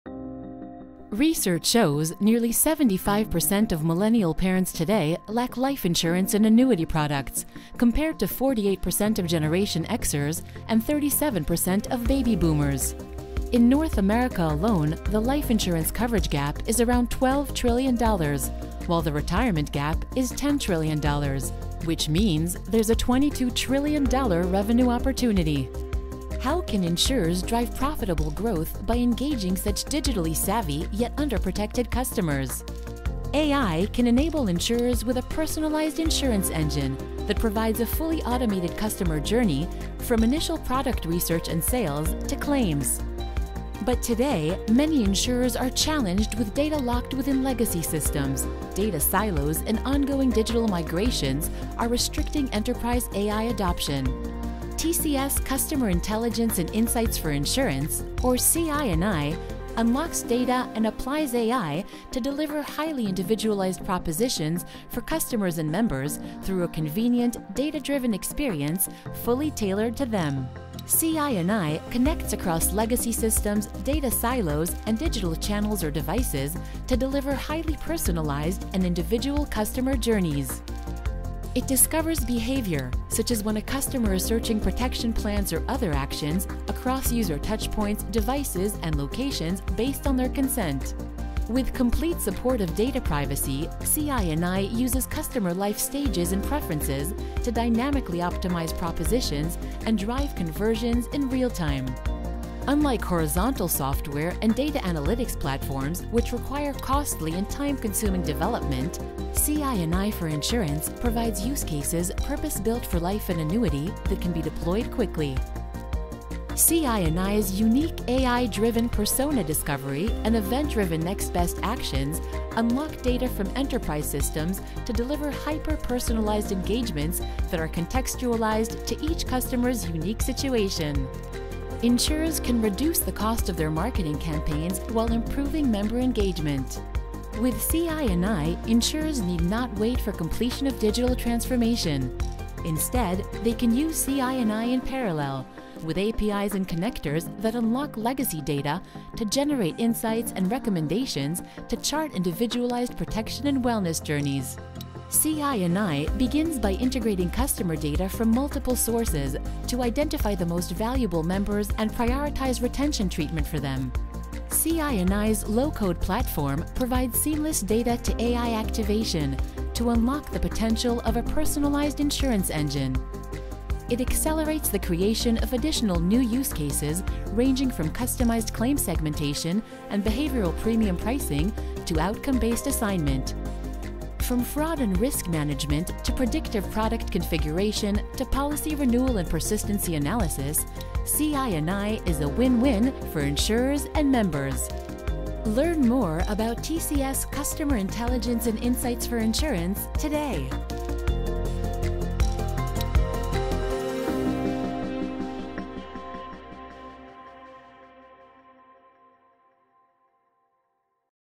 Deliver personalized protection & wellness with TCS Customer Intelligence & Insights™ for Insurance
Discover how TCS’ customer analytics software built for insurers- Customer Intelligence & Insights™ for Insurance (CI&I) unlocks data and applies AI to deliver highly personalized products and experiences that drive deeper customer engagement, satisfaction and profitable growth. From fraud and risk management to predictive product configuration, to policy renewal and persistency analysis, TCS’ CI&I is a win-win for Insurers and members.